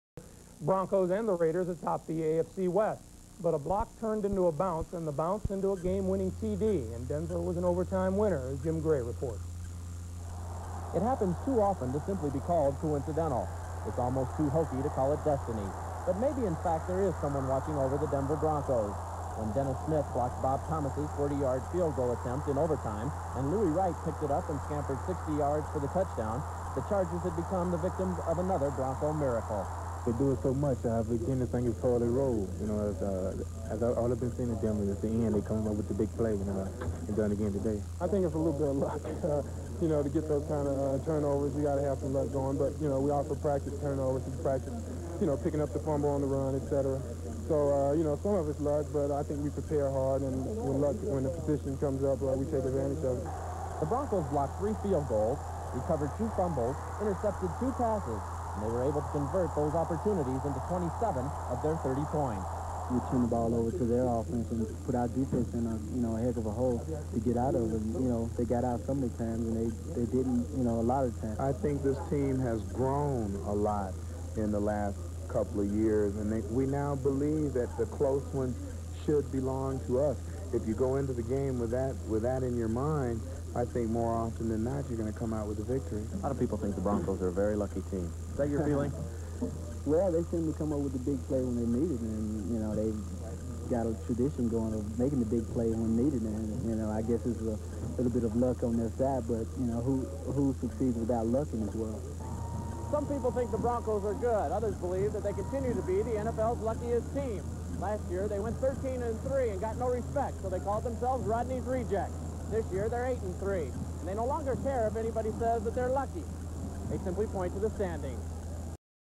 San Diego Chargers vs Denver Broncos (November 17, 1985) "Louis Wright Scoops/Scores Winner In OT"
(Time: 2:00)  Jim Gray (ESPN)